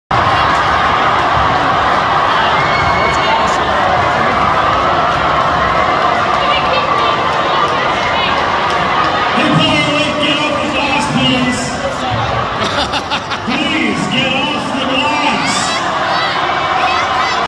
District 2AA Hockey Final - "Prior Lake, get off the glass please!"
Let's hope Alex Bump didn't hear the announcer!

Prior Lake Lakers varsity hockey team celebrating their win.. go Lakers at state!